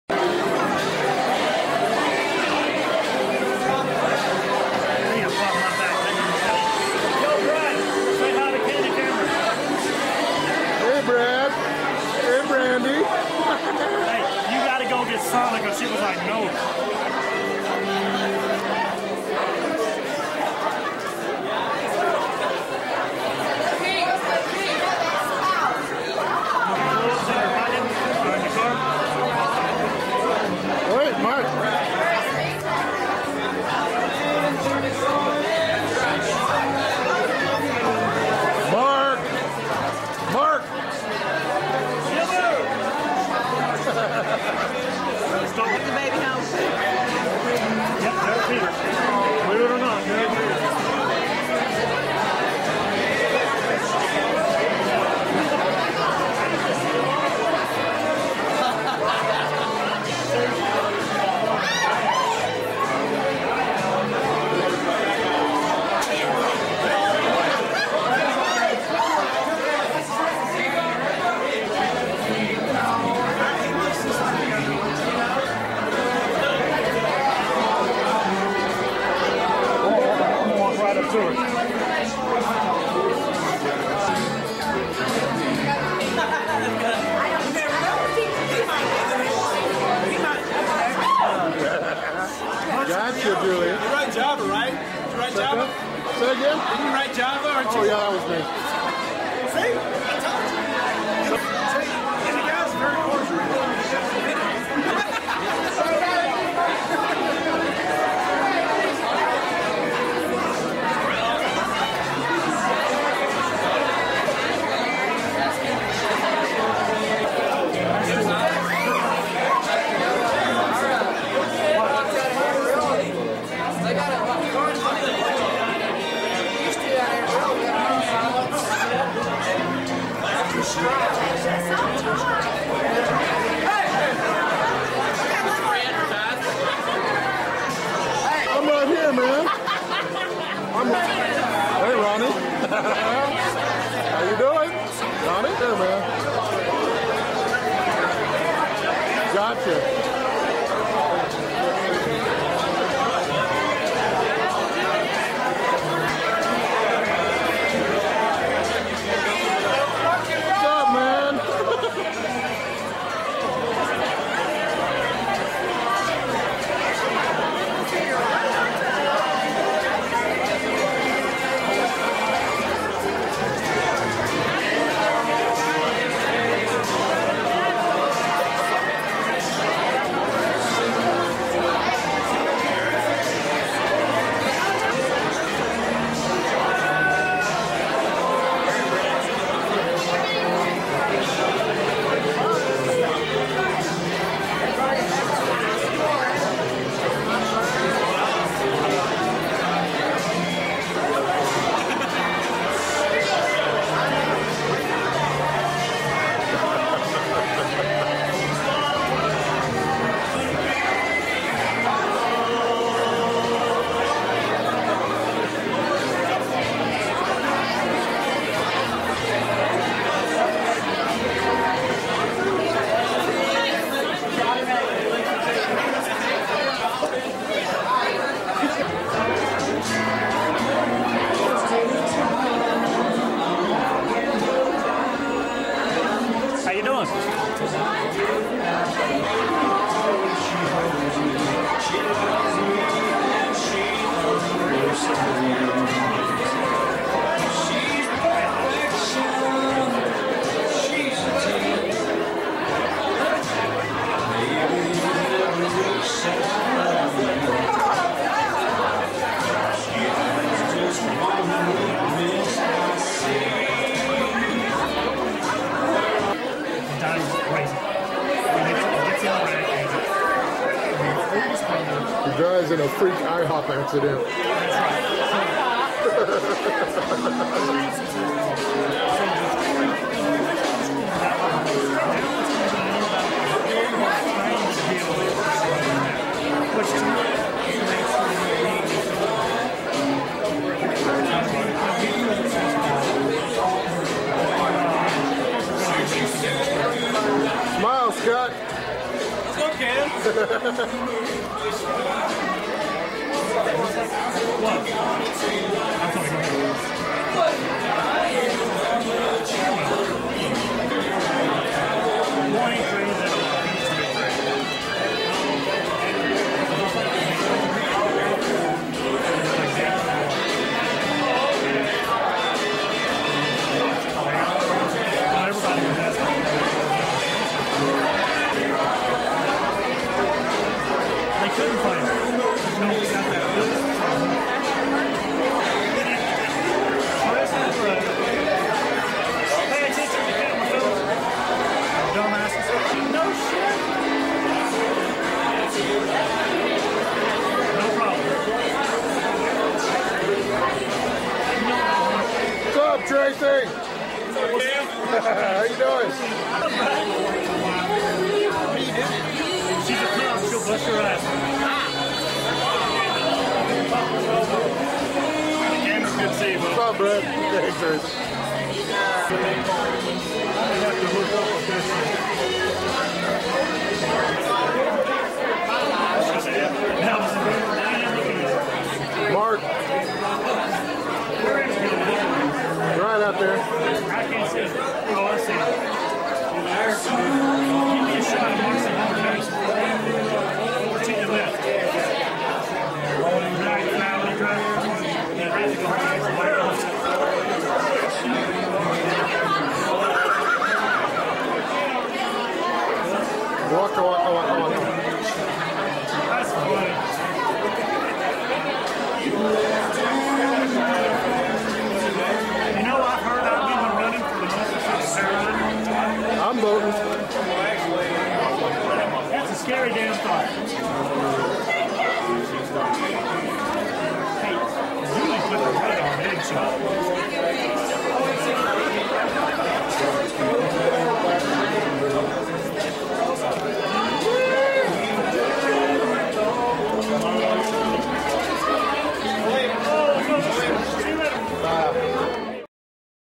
001
Pampa High School Class of '88 Ten Year Reunion - Alright, I'm going to go ahead and blame most of this on Tommy Brewer and my general state of drunkenness. Apologies in advance to any and all who are offended. Can anyone even remember what being 28 felt like?